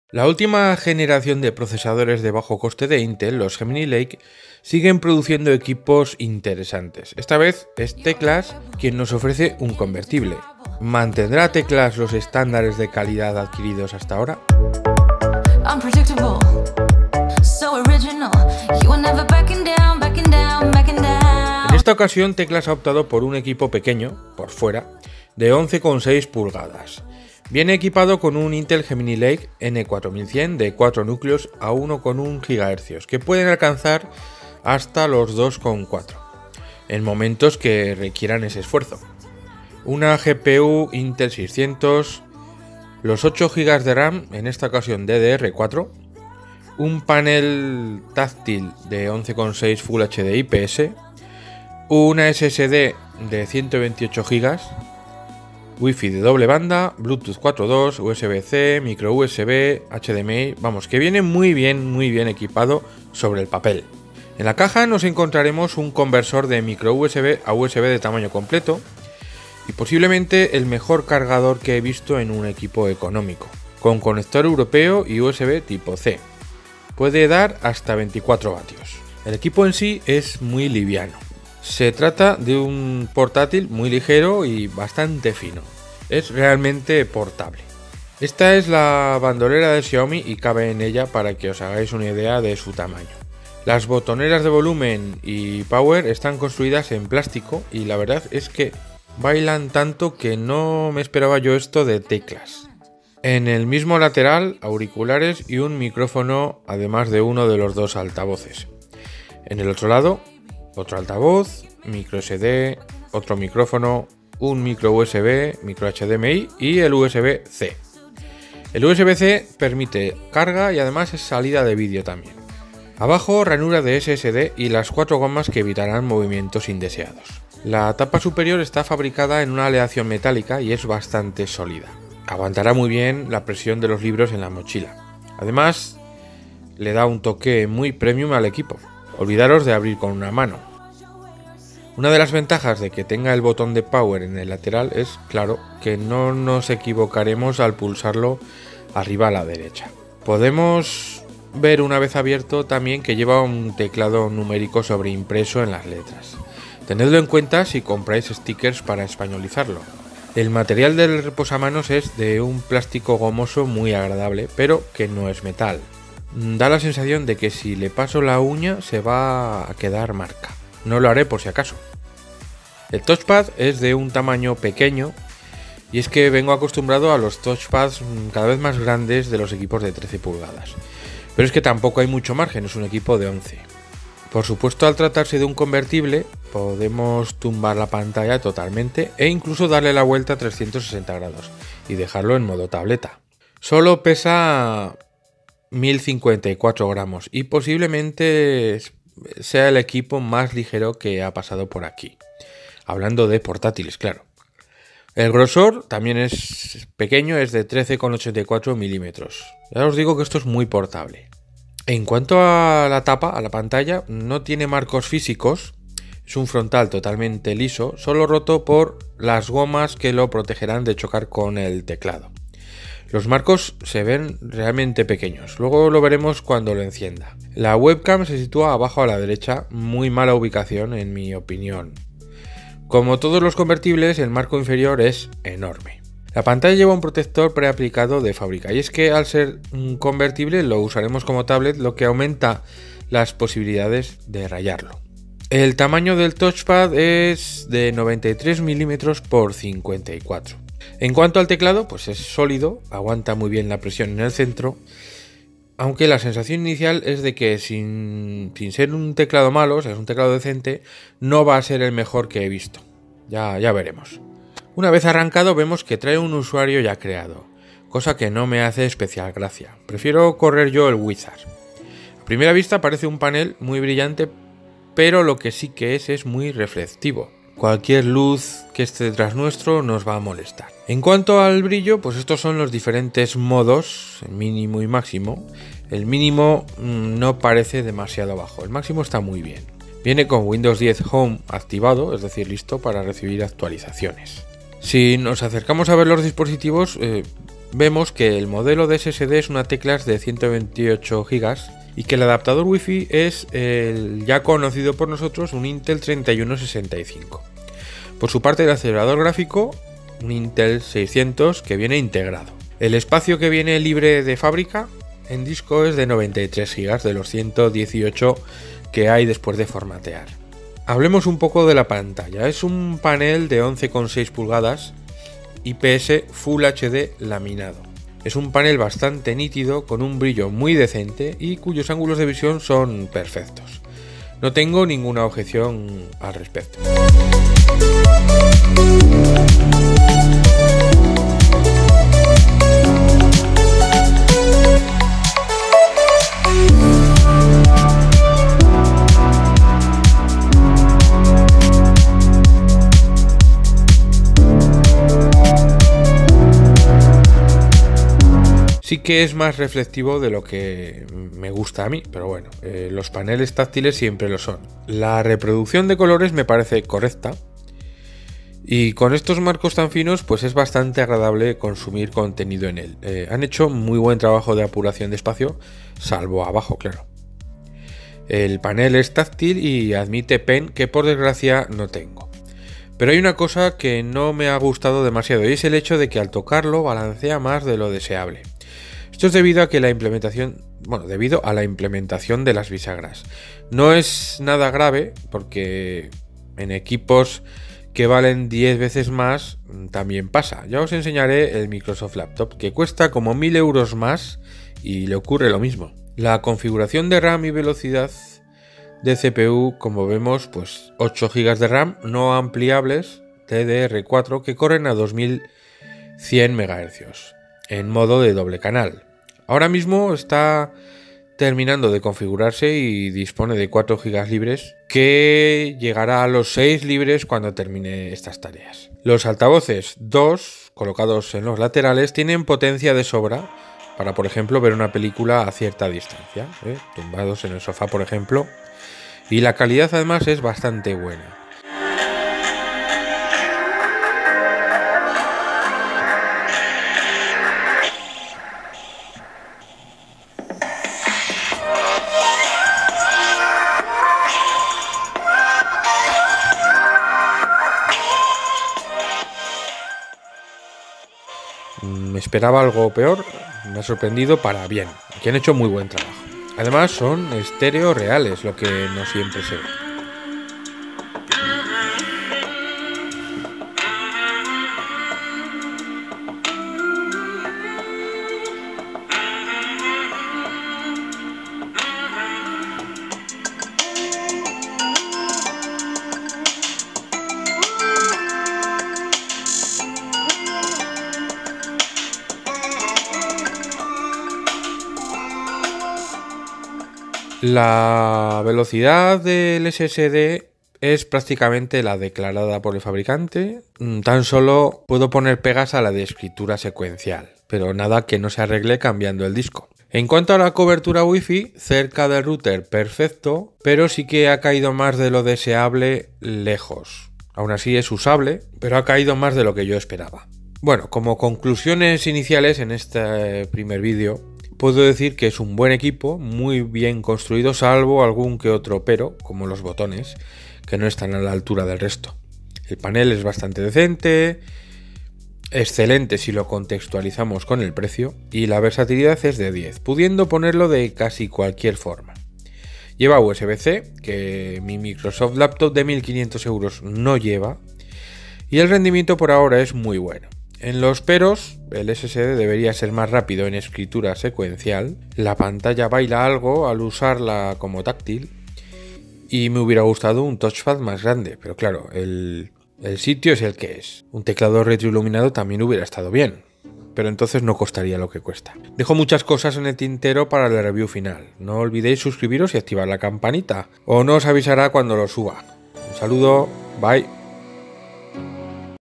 Teclast F5 unboxing y pre-review: 8 Gb de Ram para moverlo todo
Posiblemente el modelo más pequeño con los nuevos Gemini Lake, Teclast nos trae su visión de convertible.